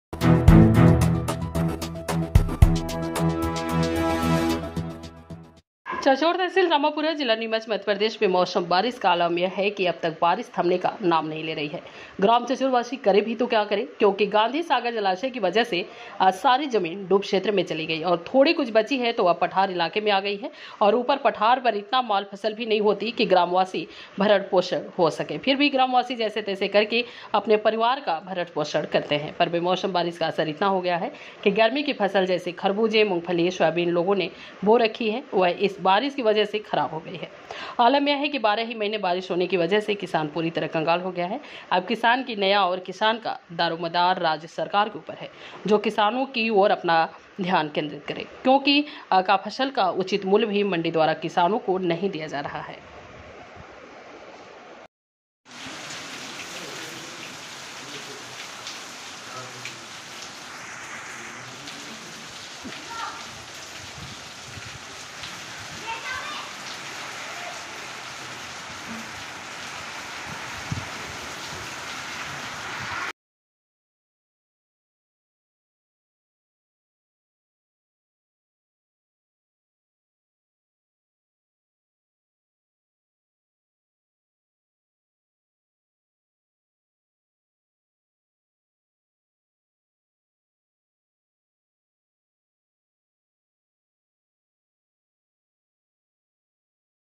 ग्राम चचोर में औलो के साथ आंधी तूफान के साथ हुई भारी बारिश#viral
ग्राम चचोर में औलो के साथ आंधी तूफान के साथ हुई भारी बारिश

चचौर में हो रही है भारी बारिश,,,,,,,,,,,,,,,,,,, चचौर से दशरथ माली की रिपोर्ट,,,,,,,,,,,,,,,, चचौर तहसील रामपुरा जिला नीमच मध्य प्रदेश बेमौसम बारिश का आलम यह है कि अब तक बारिश थमने का नाम नहीं ले रही हैं ग्राम चचोर वासी करे तो भी क्या करें क्योंकि गांधी सागर जलाशय की वजह से सारी जमीन डूब क्षेत्र में चली गई और थोड़ी कुछ बच्ची है तो वह पठार इलाके में आ गई हैं और ऊपर पठार पर इतना माल फसल भी नहीं होती है कि ग्राम वासि भरण पोषण हो सके फिर भी ग्रामवासी जैसे तैसे करके अपने परिवार का भरण पोषण करते हैं पर बेमौसम बारिश का असर इतना हो गया है कि गर्मी की फसल जैसे खरबूजे , मूंगफली, सोयाबीन लोगों ने बो रखी है वह इस बारिश की वजह से खराब हो गई हैं आलम यह है कि 12 ही महीने बारिश होने की वजह से  किसान पूरी तरह कंगाल हो गया है अब किसान की नैया और किसान का दारोमदार राज्य सरकार के ऊपर हैं जो किसानों की ओर अपना ध्यान केंद्रित करे ।।क्योंकि का फसल का उचित मूल्य भी मंडी द्वारा किसानों को  नहीं दिया जा रहा है